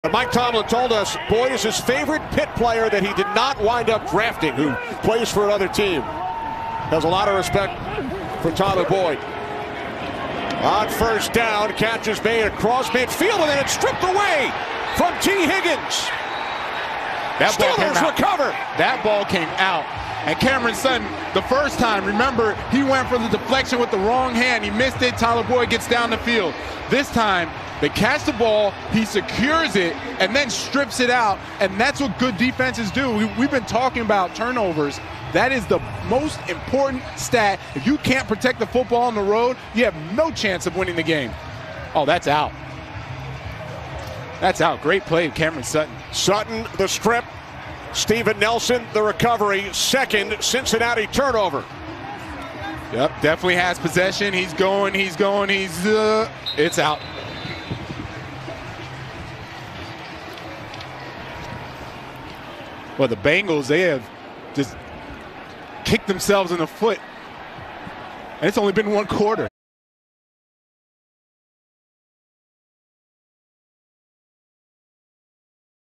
Cameron Sutton's HUGE Strip on Tee Higgins
After a catch by Tee Higgins, Cameron Sutton muscles it away from him to give the Steelers the ball back. The Cincinnati Bengals take on the Pittsburgh Steelers during Week 10 of the 2020 NFL season.